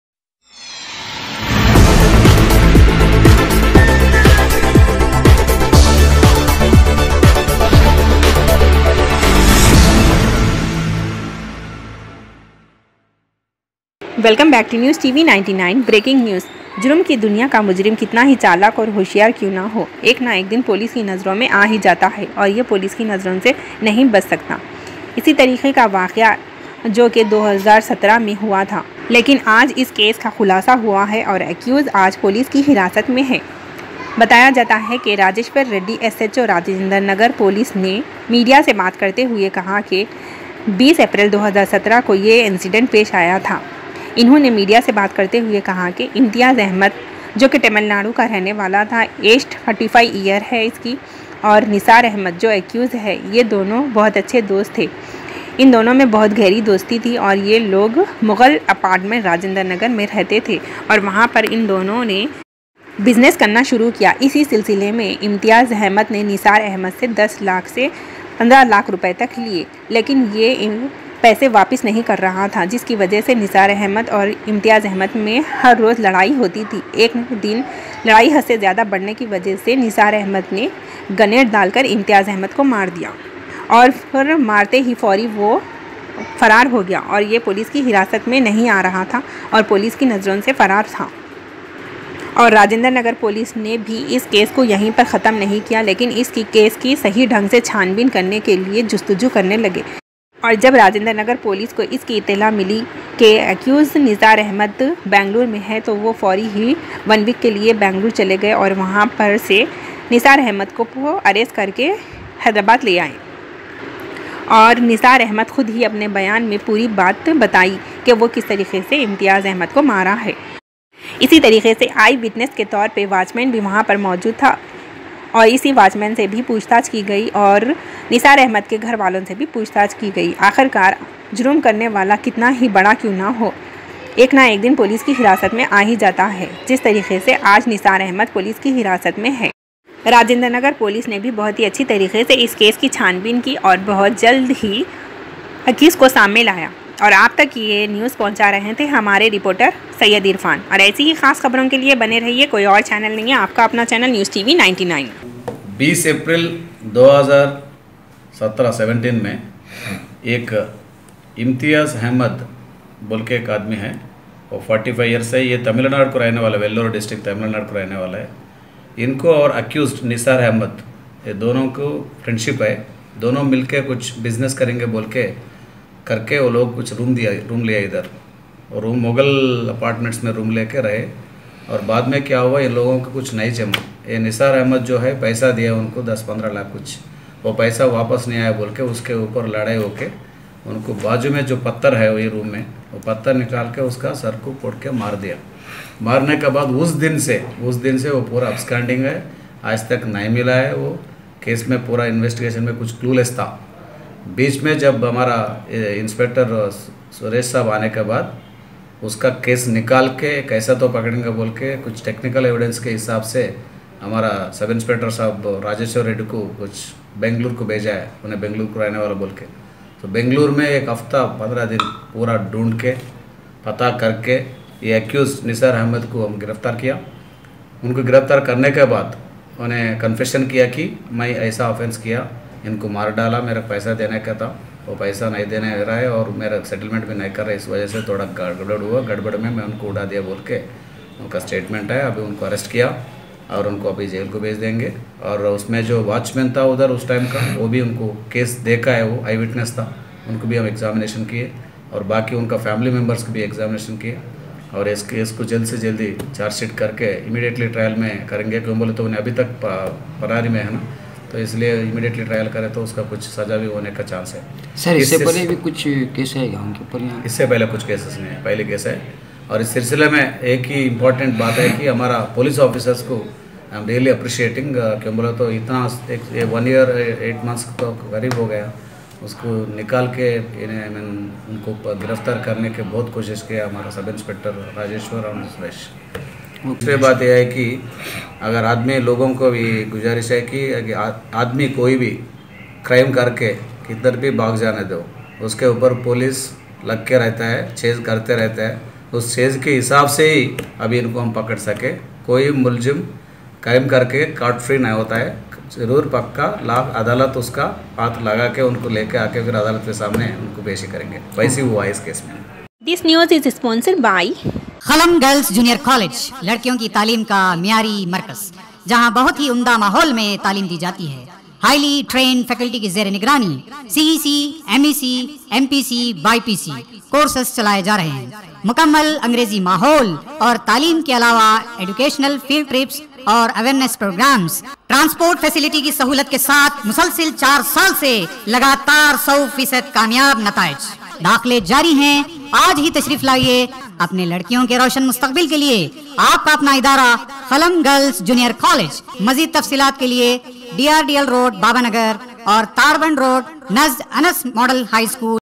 RAJENDRA NAGAR P.S|MURDER CASE LONG PENDING ARRESTED ACCUSED NISAR AHMEDAR IN 16 MONTHS AGO..
RAJENDRA NAGAR POLICE STATION|MURDER CASE LONG PENDING ARRESTED ACCUSED  NISAR AHMED IN 16 MONTHS AGO